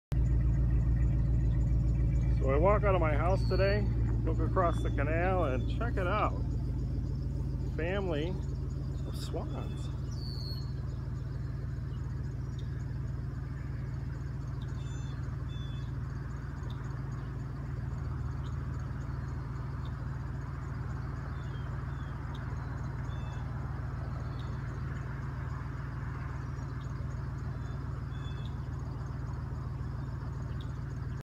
The boat house swan visit
The boat house swan visit, I walk outside and what do I see. A whole family of swans nesting. I LOVE WHERE I LIVE.
Formerly known as The Bridge boat bait and tackle shop for decades, now my home, the boat house. 47075 Jefferson Ave MI

Google jack Johnson Realtor